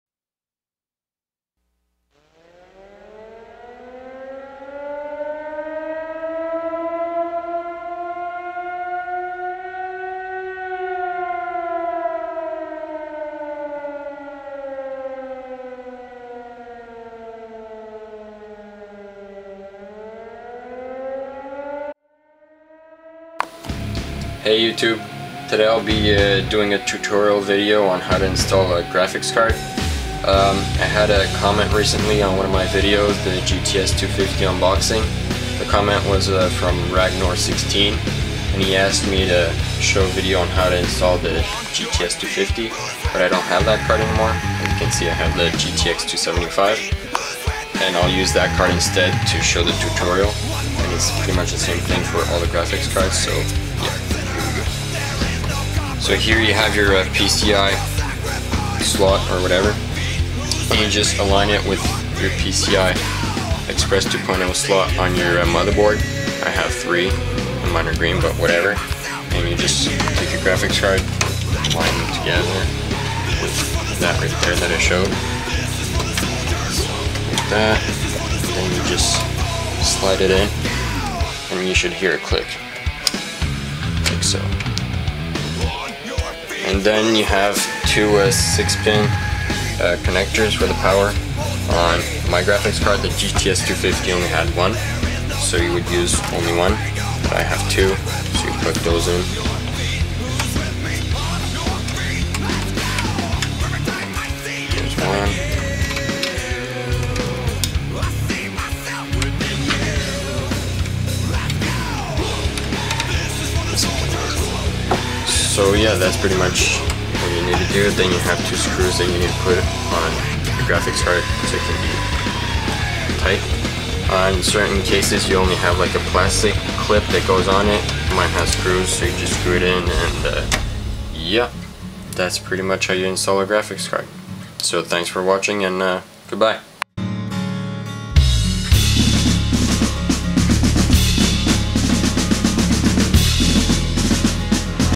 How to install a graphics card
I got a comment from raggnor16 on how to install the gts 250 but i don't have that card any more so i used my gtx 275 Enjoy! And subscibe.